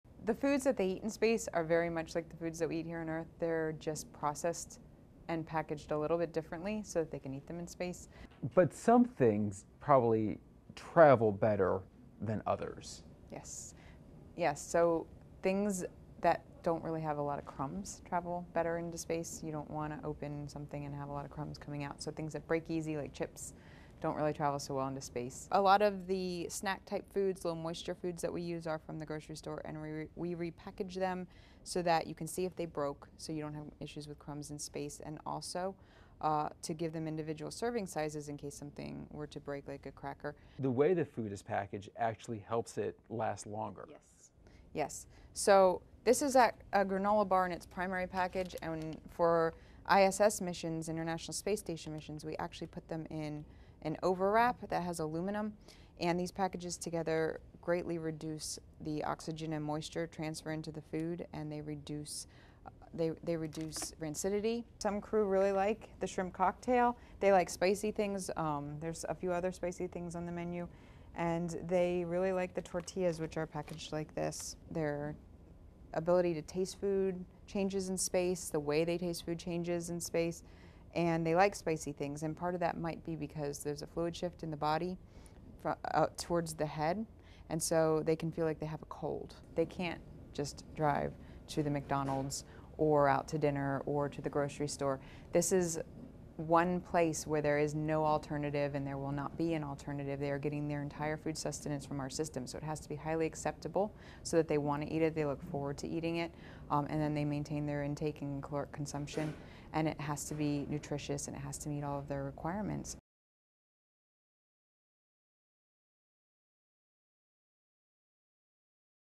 Space foods need to be specially prepared
NASA food scientist Dr. Grace Douglas explains the specially prepared or packaged foods that Coloradan astronaut Steve Swanson will eat during his visit to the International Space Station.